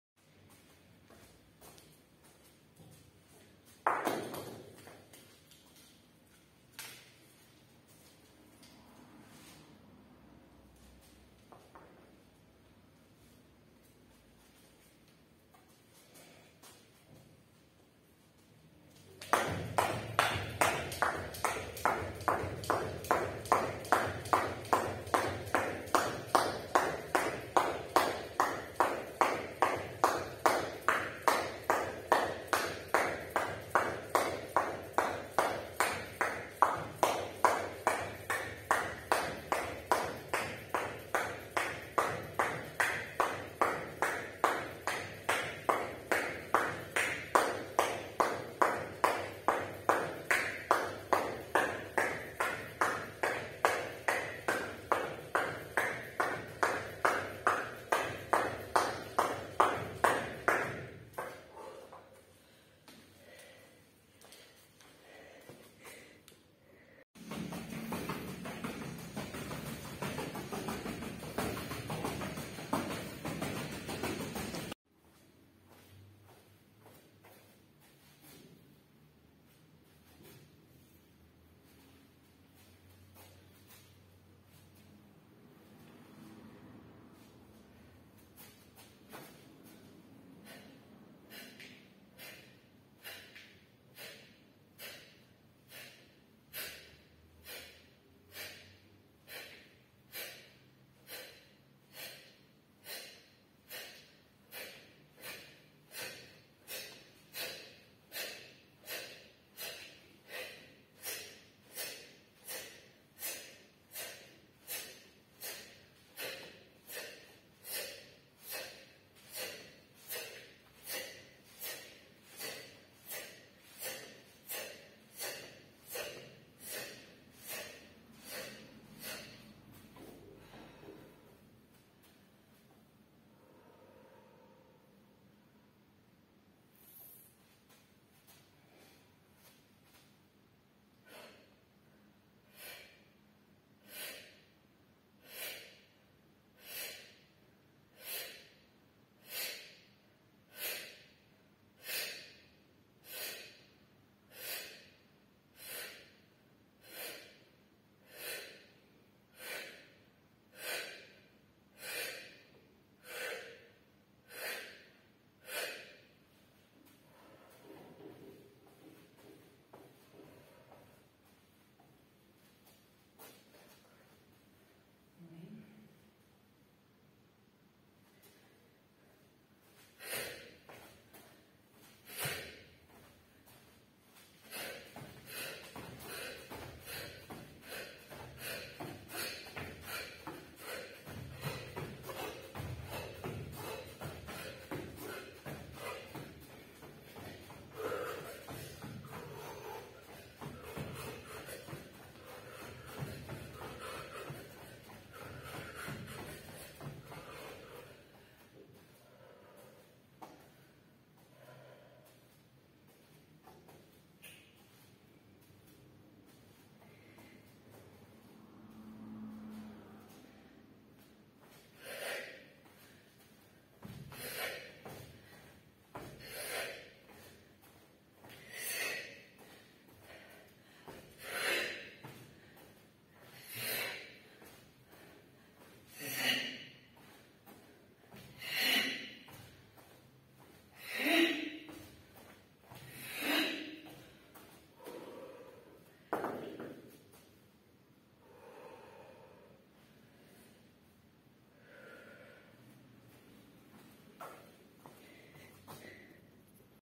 Sport. Warming-up and exercises with effort. 🏃
Sports exercises in the gym and at home.
👆Who counts jumping rope?)
👆Who does the similar exercises?
-Share information in the comments. 🤗❤️ Thanks in advance.
👌 Have a nice day and excellent mood!
Sincerely yours, Alla Symonenko.